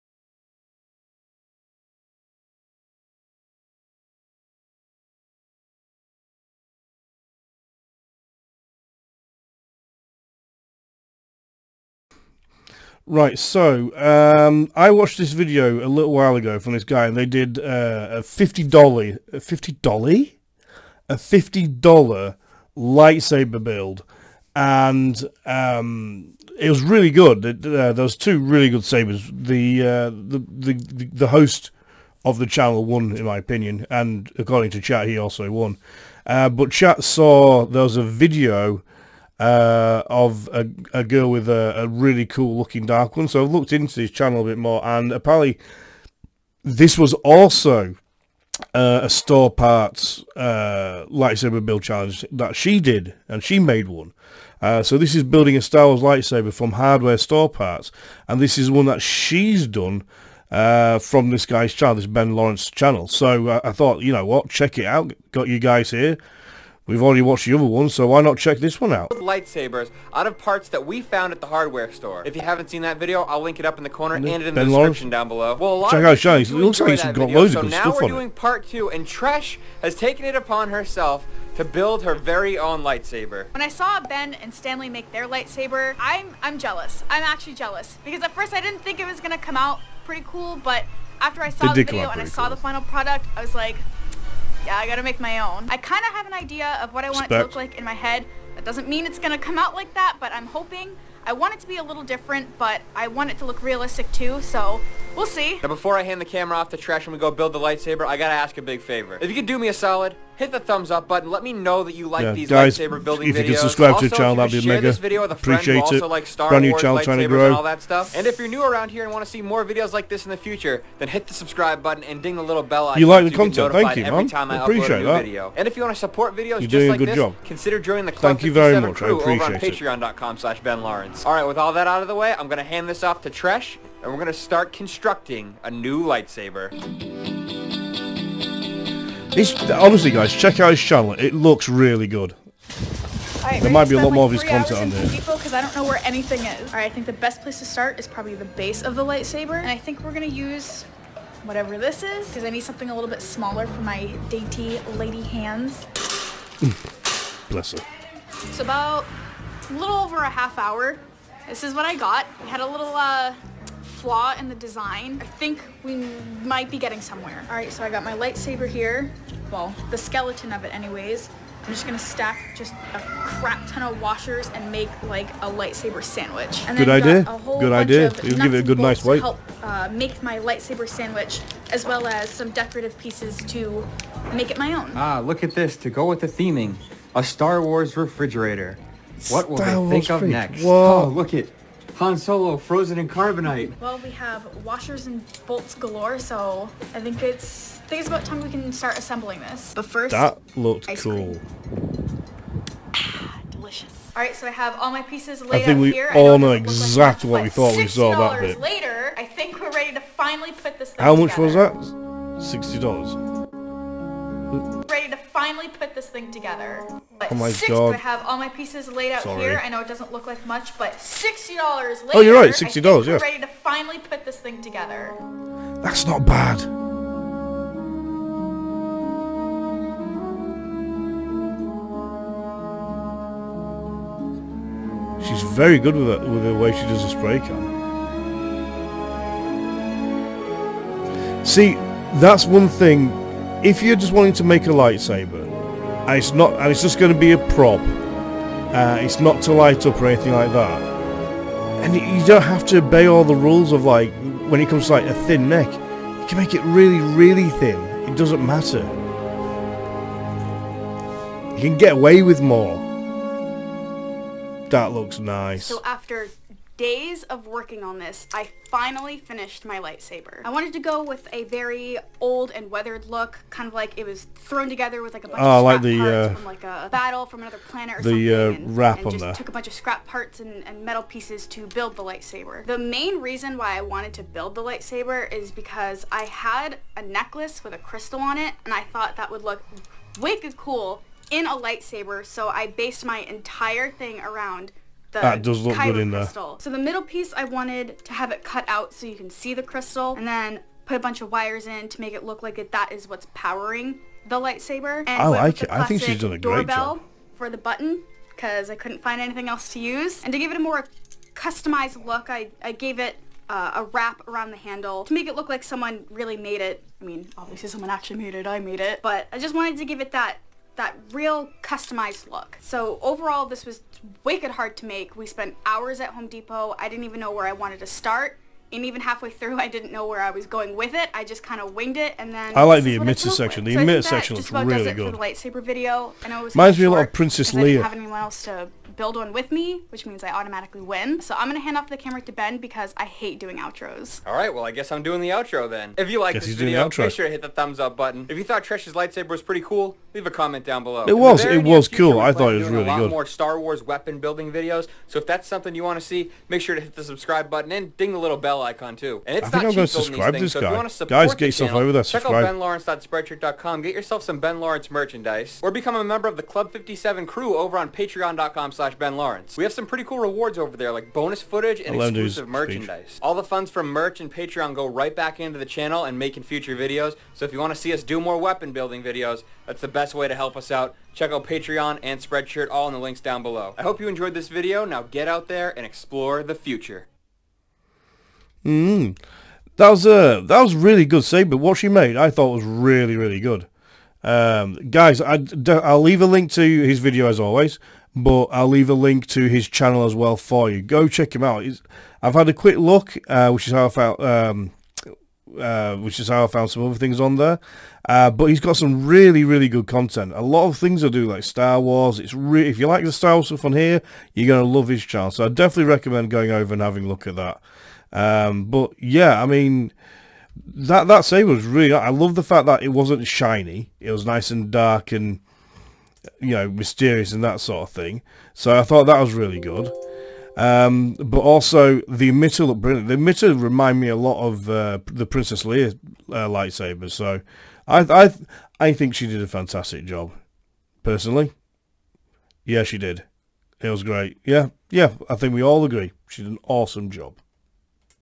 Making a STAR WARS Lightsaber from Hardware Parts | BenSabers Reacts
It's just me and my streaming viewers here so it's apprecaited.